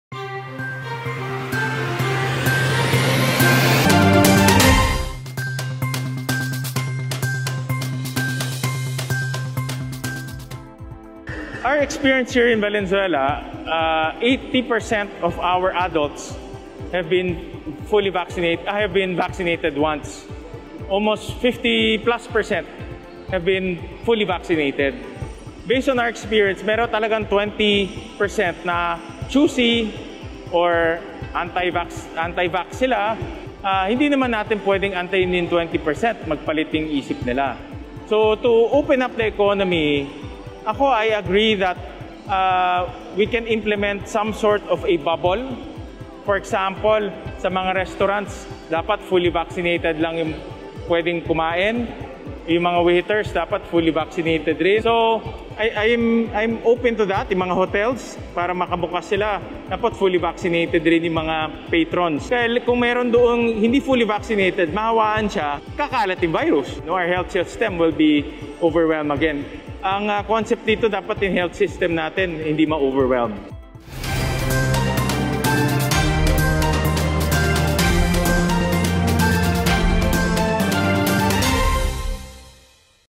Balitang WIN (September 23, 2021)
Para hindi magkahawaan, dapat bakunado ang ating mga kababayan na nagtatrabaho.